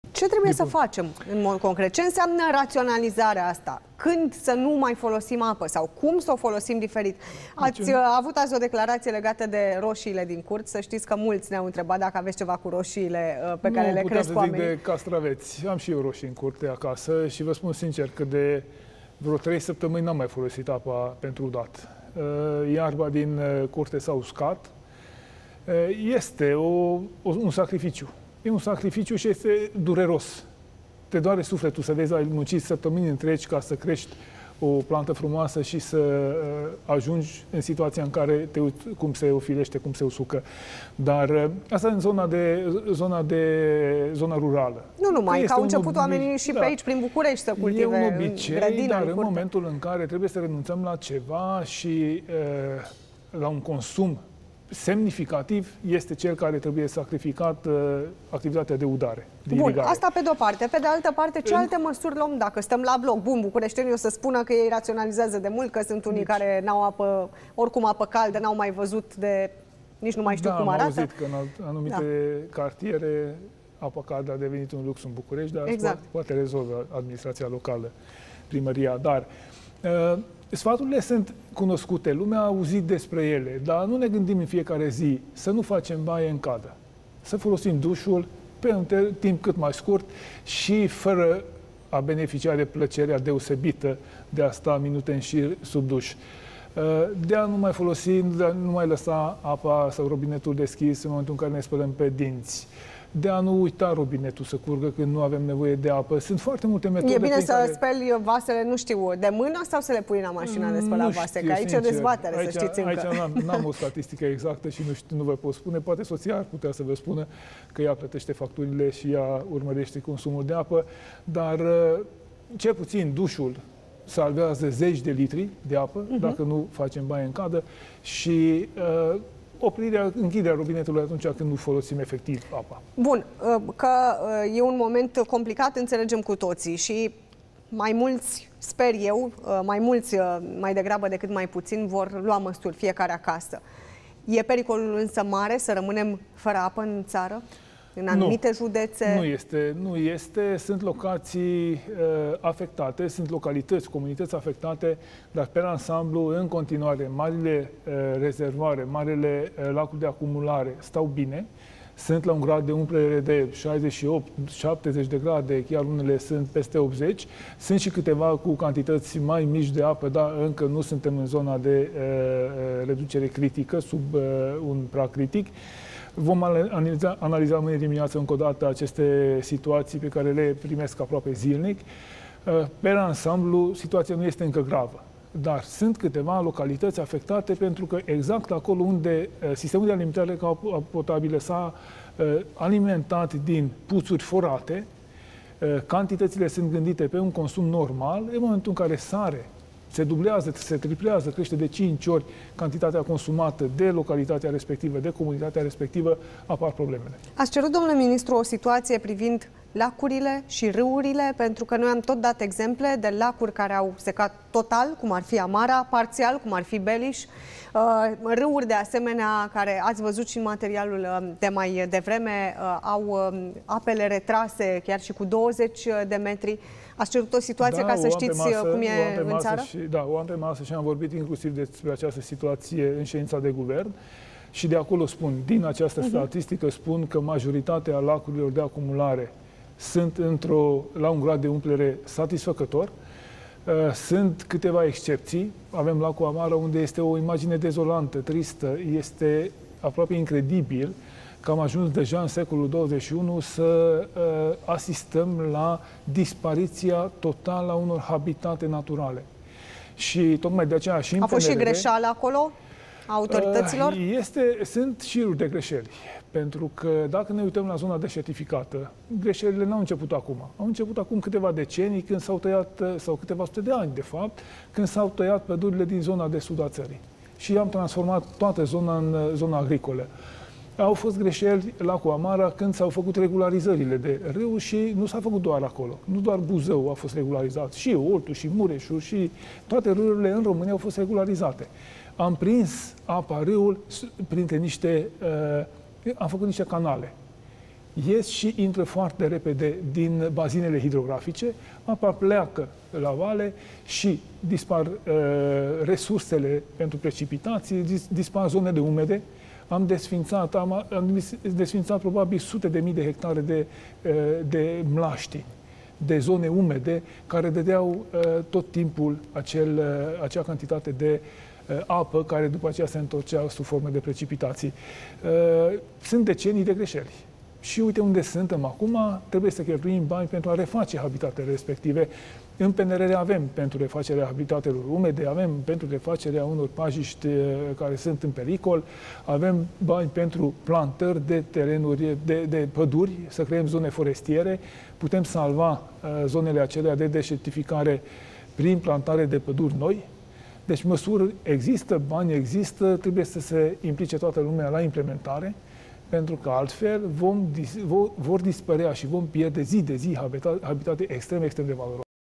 Ministrul Mediului, sfaturi pentru români pentru a raţionaliza consumul de apă
Ministrul Mediului, Tanczos Barna, face apel la populaţie să utilizeze conștient apa potabilă şi a explicat dacă România se află în situaţia de a rămâne fără rezerve, în contextul extinderii caniculei şi secetei pedologice.